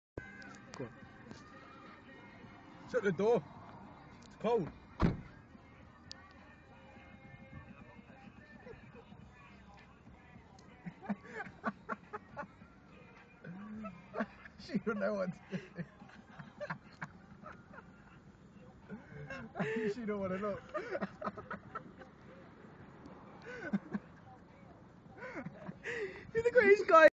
Thumbs up traffic
Teddy gives rush hour traffic the thumbs up